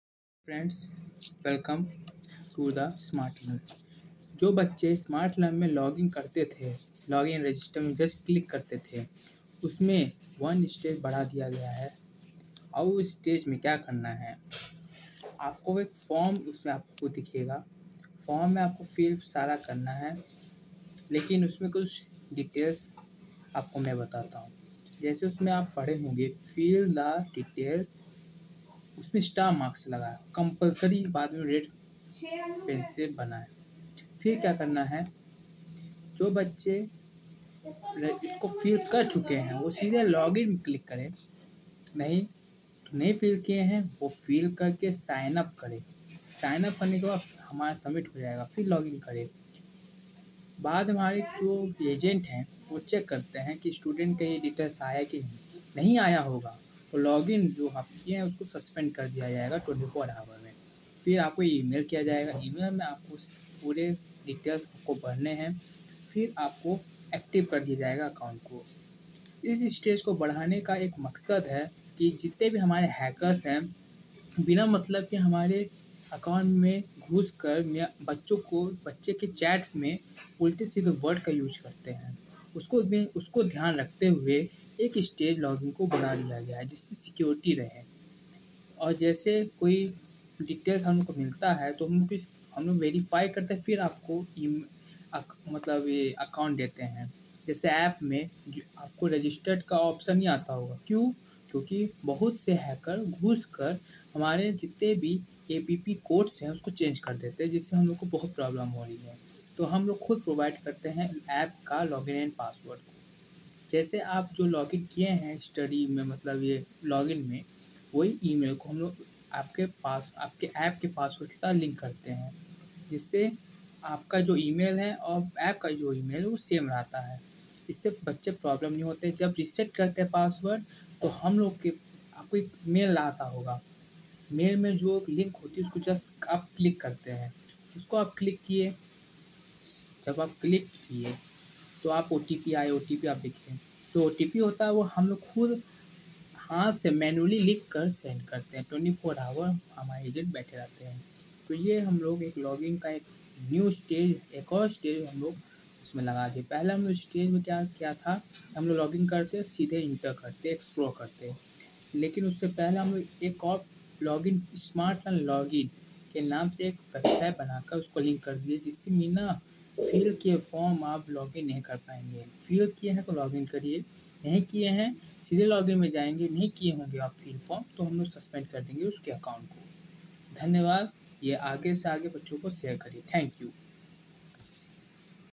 Smart Learn Build High Security Student Login in Smart Learn Website. | How To Use | By Vishveshwar
Smart Learn Company Pvt Ltd